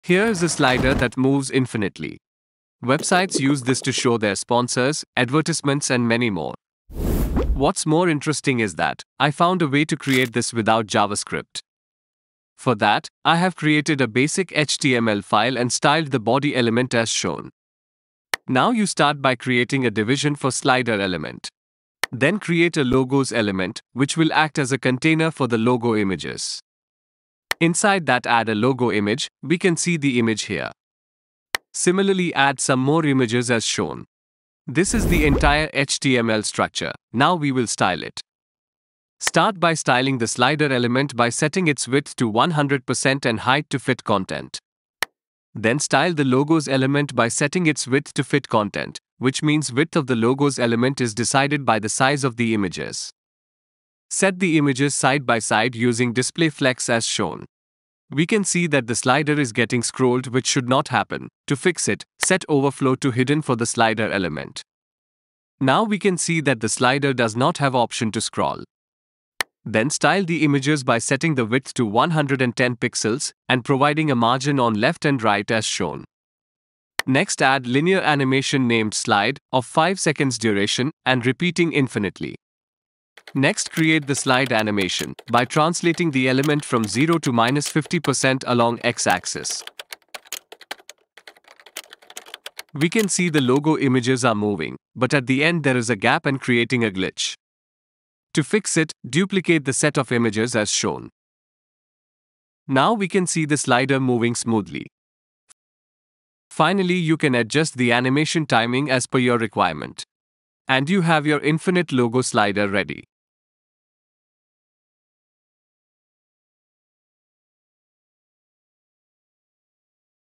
A Simple Infinite Slider EVERY Developer Should Know
We'll go through the process of building a sleek and dynamic logo slider that seamlessly loops through your logos or brand images.